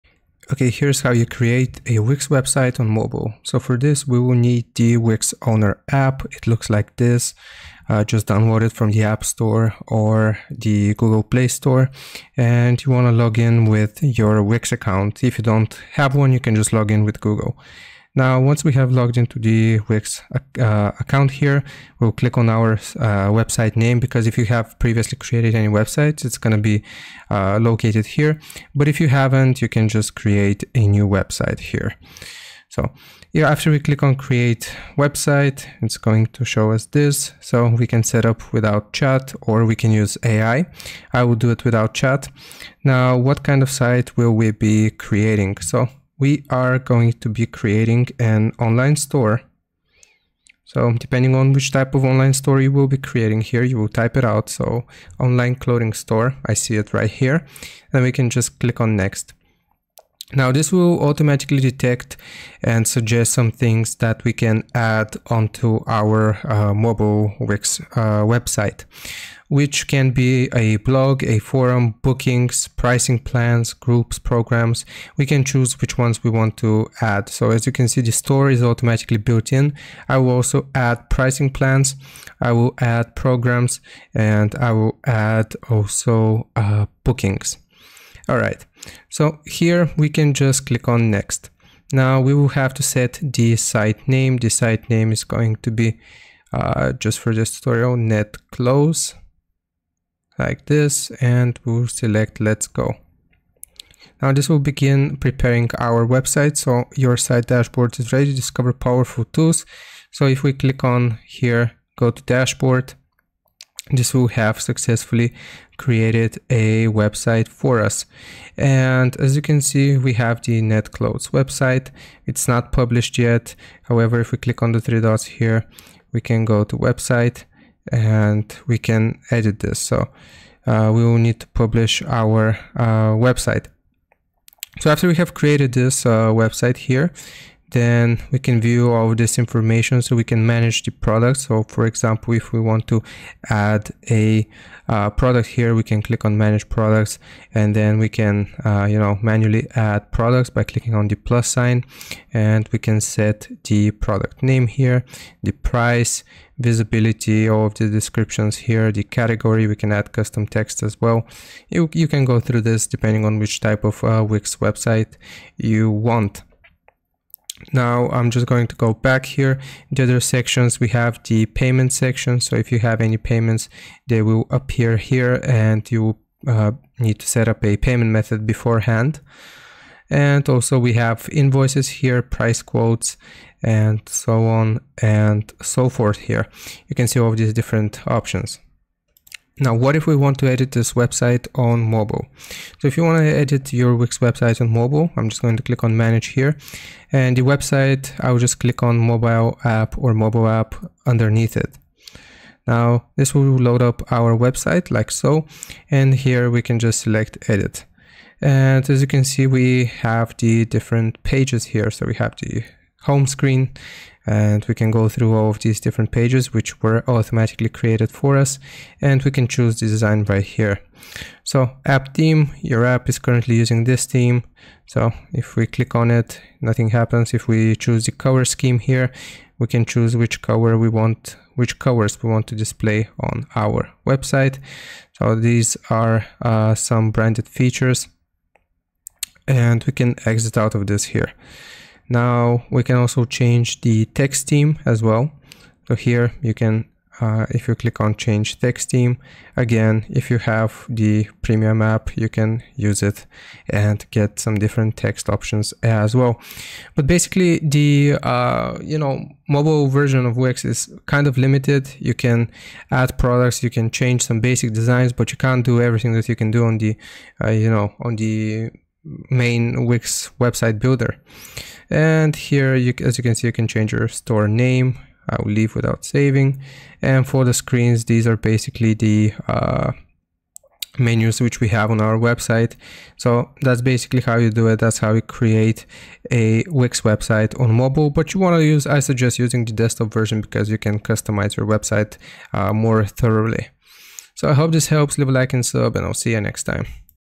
How To Create A Wix Website On Mobile 2026 (Full Tutorial)
In this video I show you how to create a Wix website on mobile in 2026. If you want to learn how to create a website using Wix on a mobile phone then this is the tutorial for you.